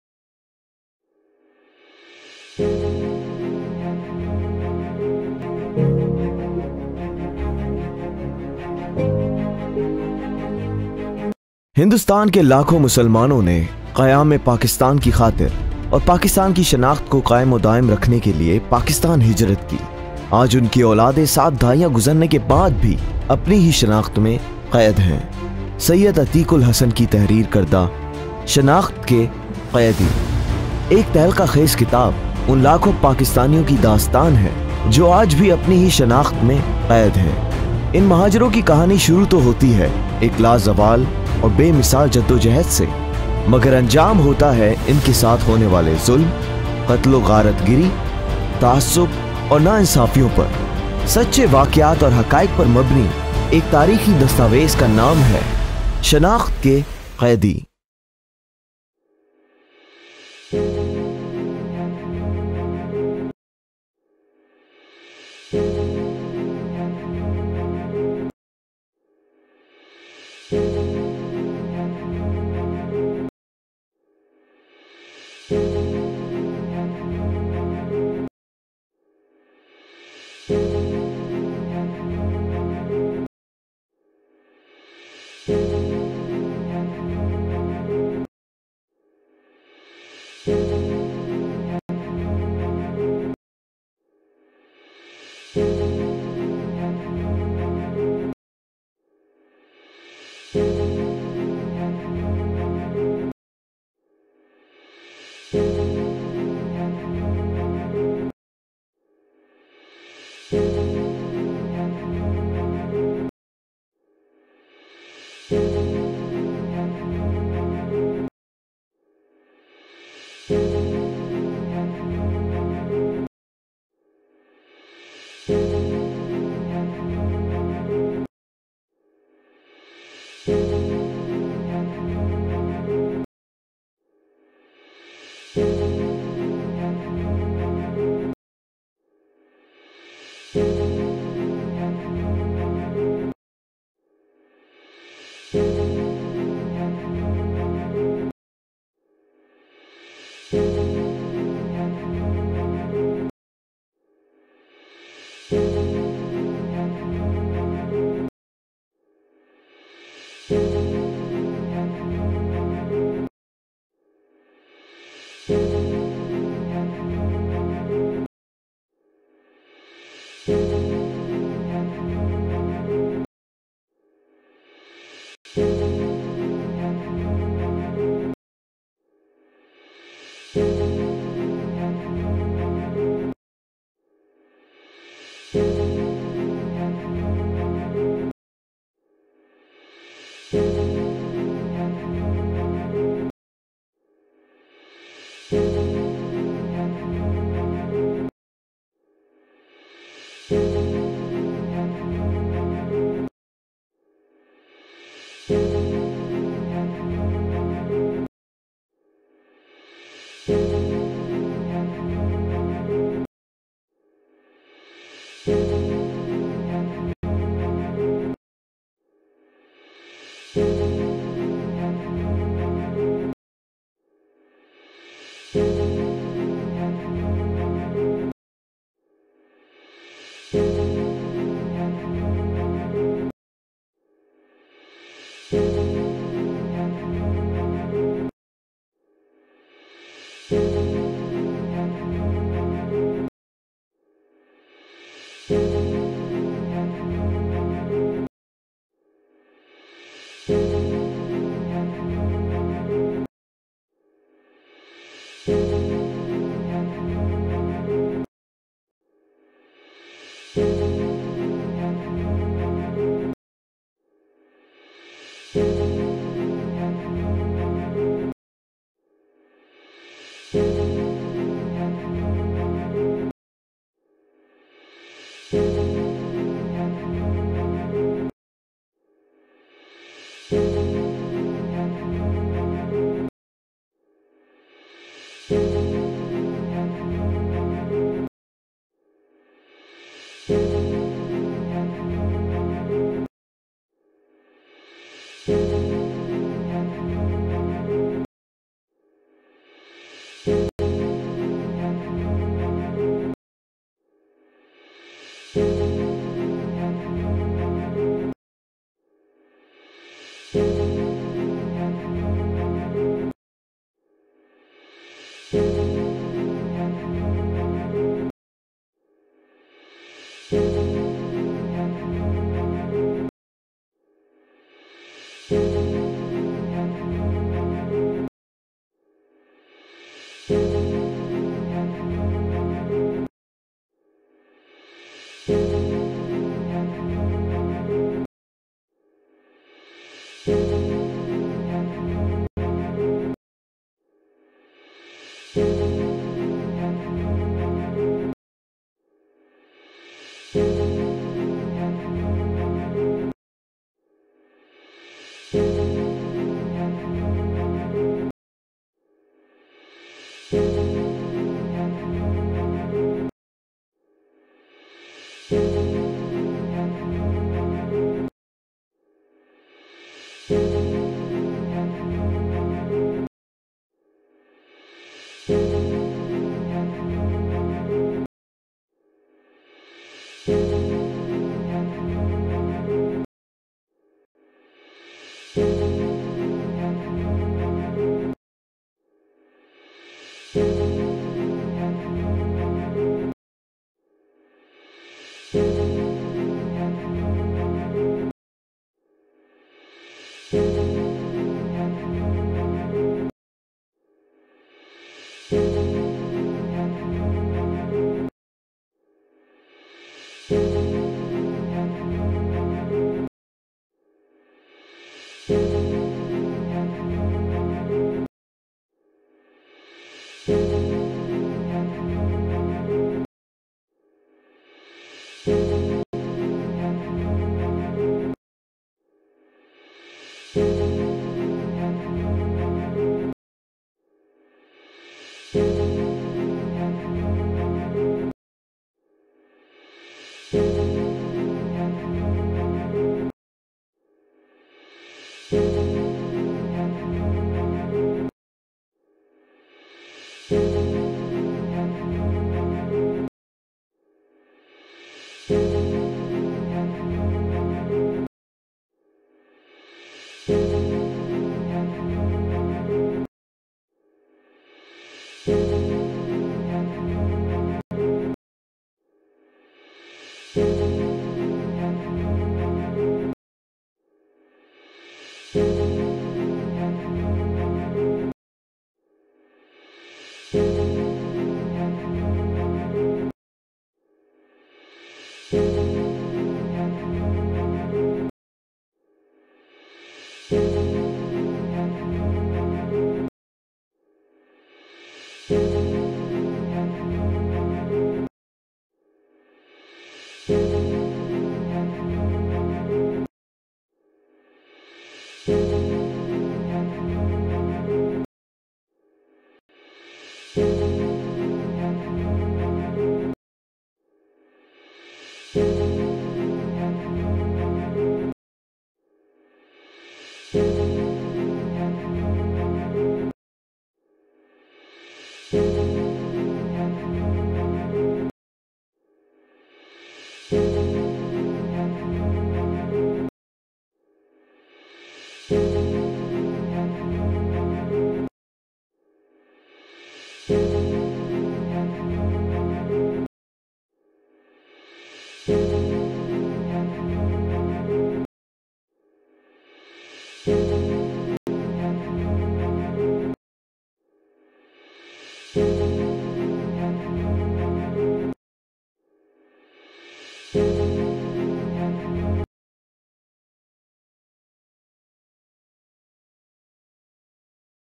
About the author of 'Shanakht key Qaidi' - Pictorial Presentation
Syed Atiq ul Hassan, is an Australian-based journalist, foreign correspondent, writer and social activist. He is author of an Urdu book 'Shanakht Kay Qaidi' which is a historical document on the discrimination & injustices of the community whose early generation migrated from India to Pakistan at the time of partition in 1947. This community is identified as Muhajirs (Urdu-speaking) in Sindh and majority of them belong to the Urban areas of Sindh, Pakistan. The author belongs to this community, was born in Hyderabad Sindh and went through personally (physically & mentally) with the discrimination through quota system and social injustice, the quota system was made part of constitution of Pakistan by the government of Zulfiqar Ali Bhutto in 1973.  He has revealed the untold stunning stories of the diaspora of Muhajirs